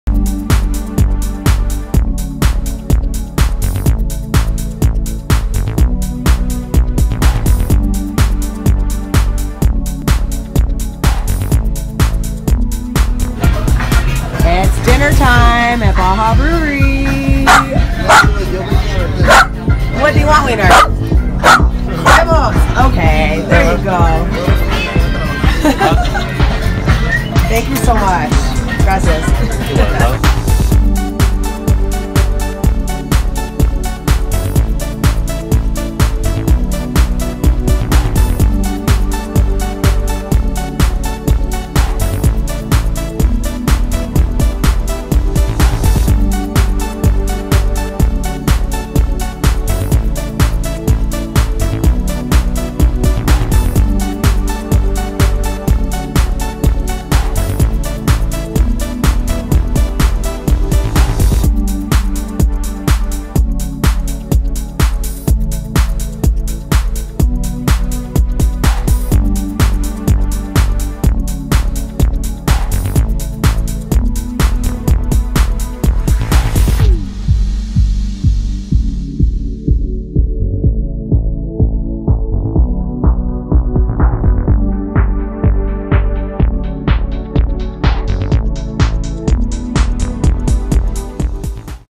Baja Brewery Downtown San Jose Del Cabo 🍺 LOVE DeeOGee!🐾
This one is our absolute favourite places! 🌮 🤩
💙 The staff there are SO great, so a friendly, so helpful, so thoughtful and they REEEEEALLY  love DeeOGee and are always giving him the full VIP treatment! 🏆